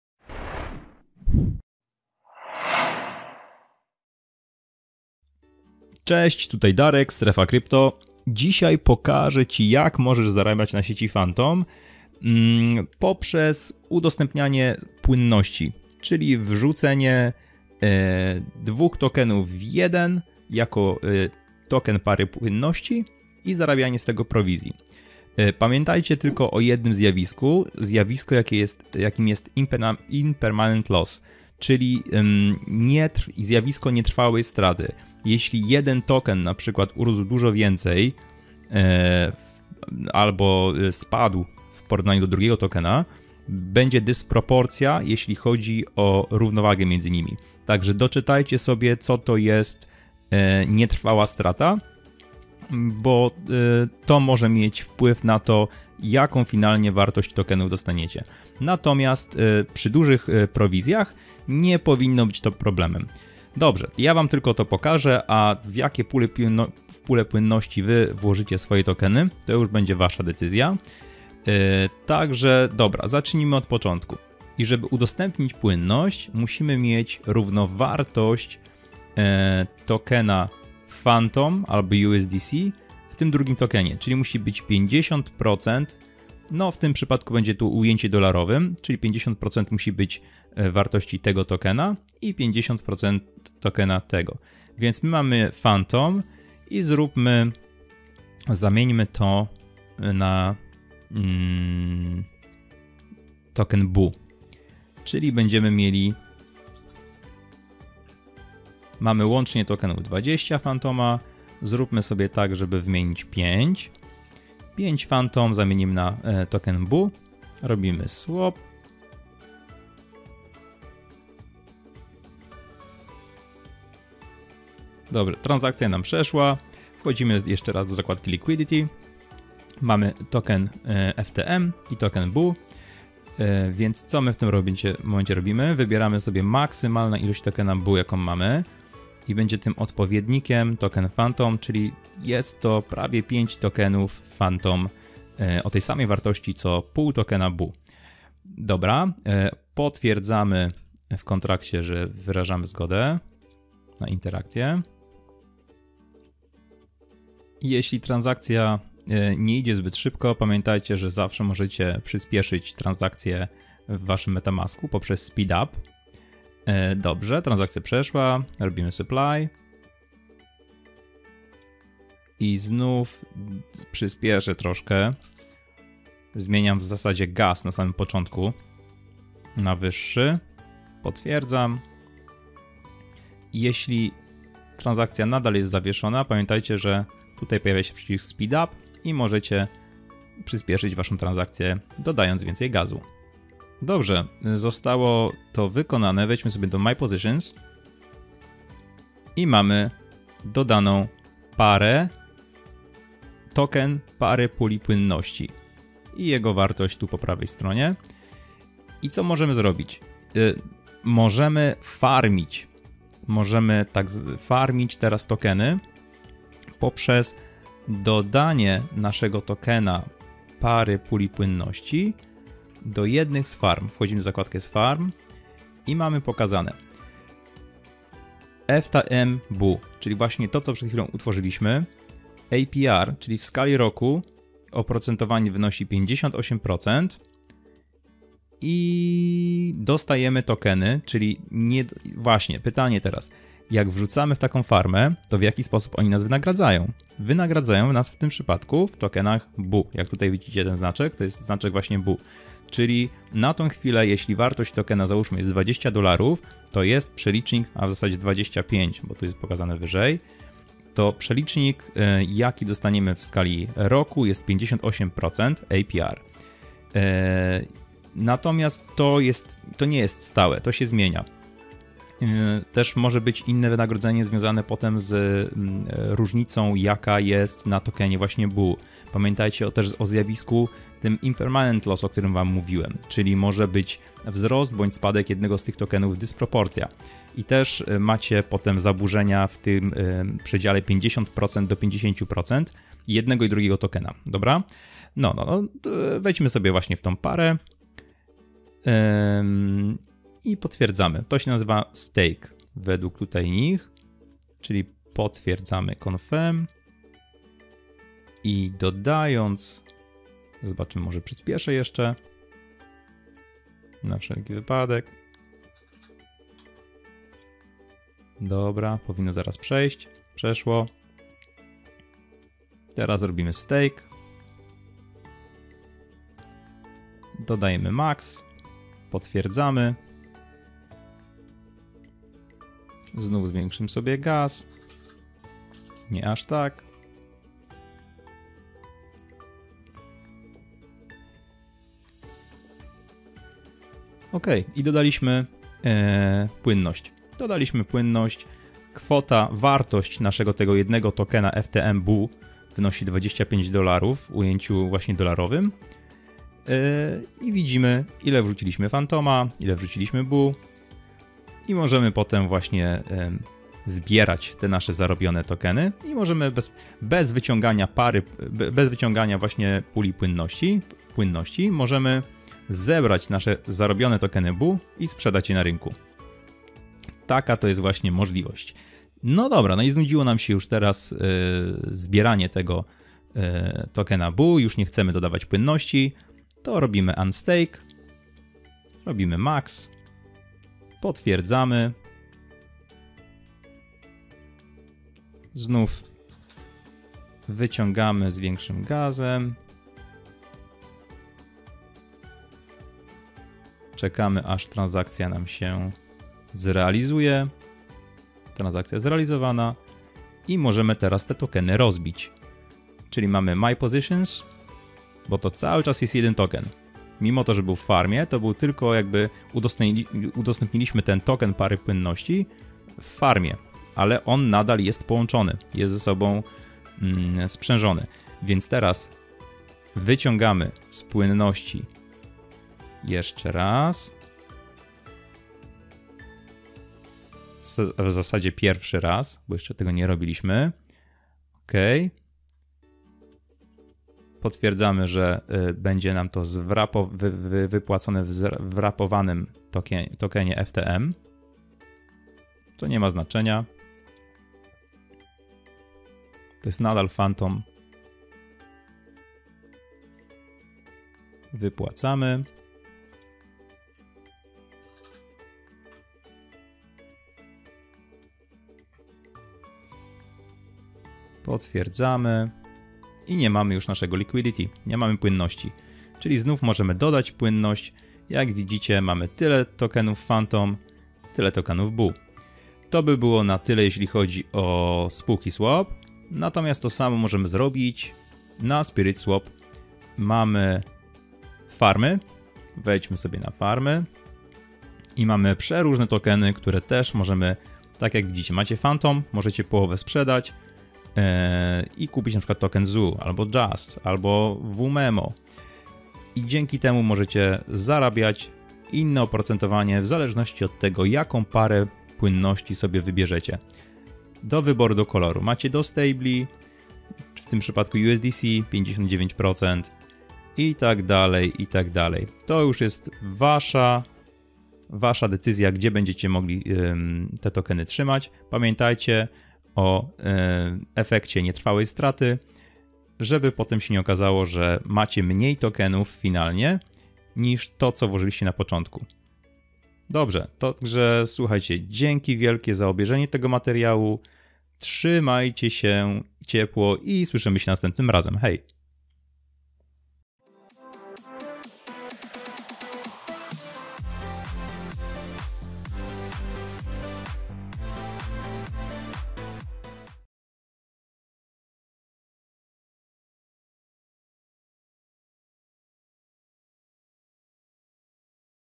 Jak zarabiać dodając płynność i farmiąc tokeny CZĘŚĆ 1. Przykład na sieci FANTOM. Uwaga na IL !!
Pokażę Ci jak dodać płynność na giełdzie dex i wrzucić token pary płynności (LP) na farmę, żeby zarabiać od kilkudziesięciu do kilkuset procent w skali roku.

Pamiętaj o zjawisku Impermament Loss (nietrwałej straty). Załączam link do opisu definicji. Przy dużym APY zjawisko 'IL' może zostać zniwelowane przez duże nagrody z farmingu.